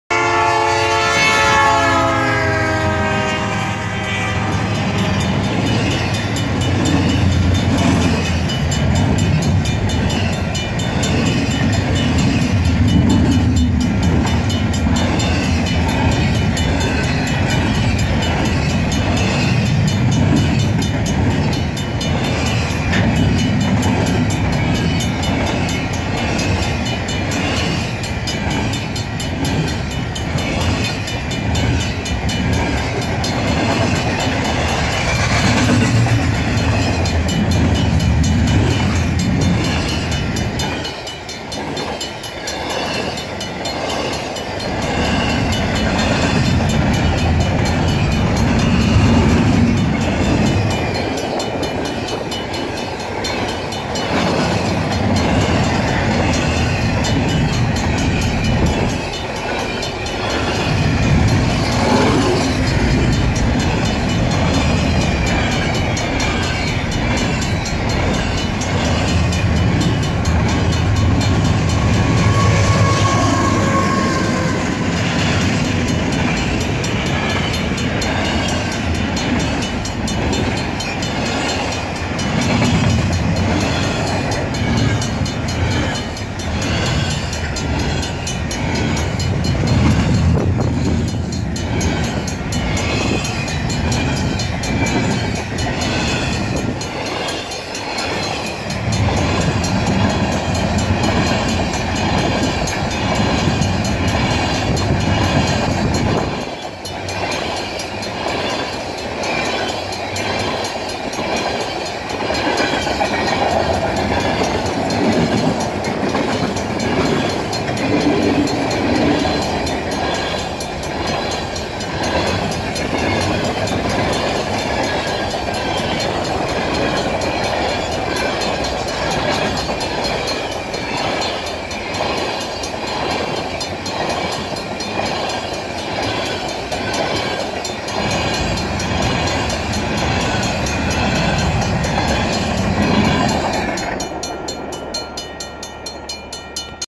Nobel Train Crossing - March 8, 2022
Just about missed the train! Got to the crossing as the train was approaching.

Canadian Pacific locomotive 8201, Kansas City Southern locomotive 4820, Canadian Pacific locomotive 8053 pulling a mixed manifest.